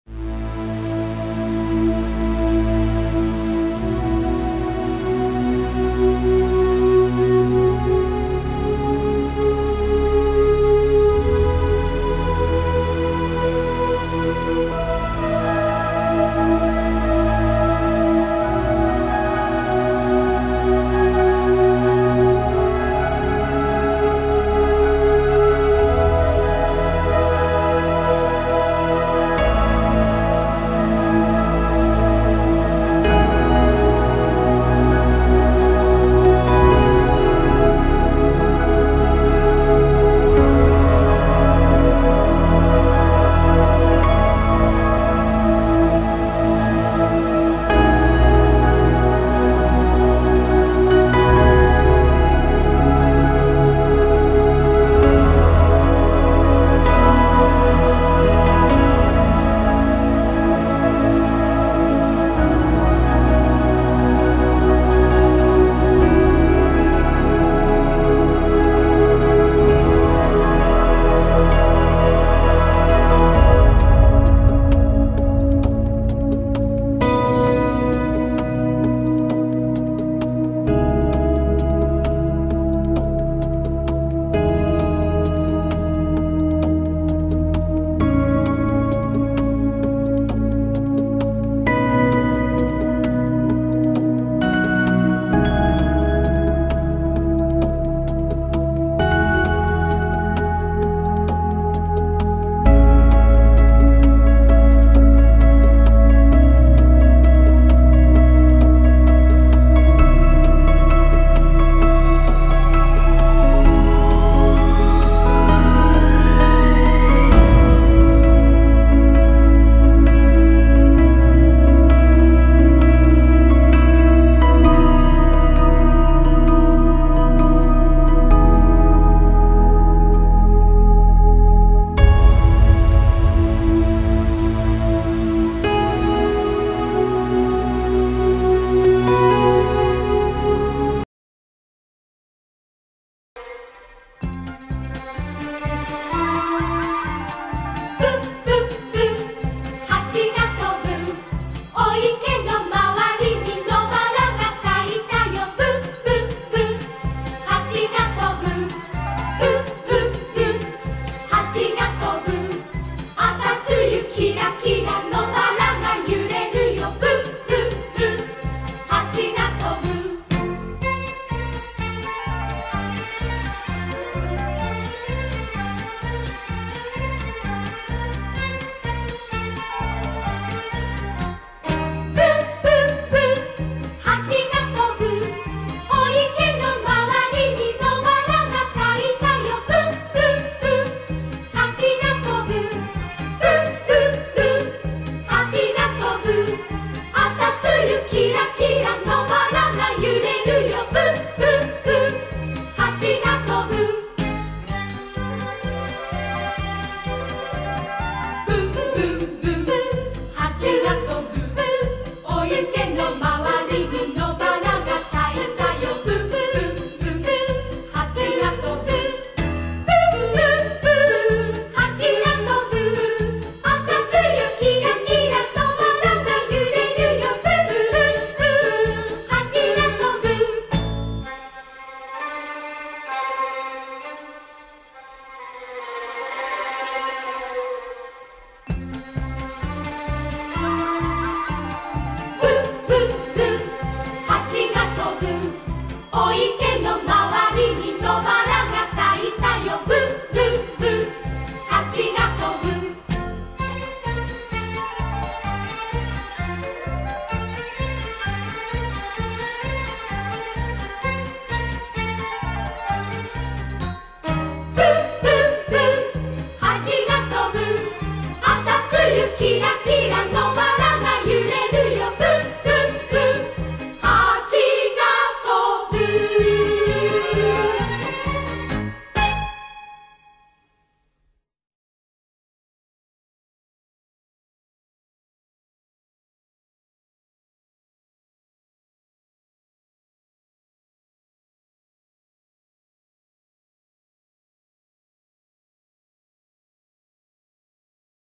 「シャクヤクの花開花」早速ミツバチがやって来ました！2021年4月21日
2021年4月21日　晴れ
シャクヤクの花が咲きました！
22日には小さなハチさんも花粉集めに余念がありません　(*^_^*)
アップで見ると、クチバシ？で花粉を採っているのが良くわかります！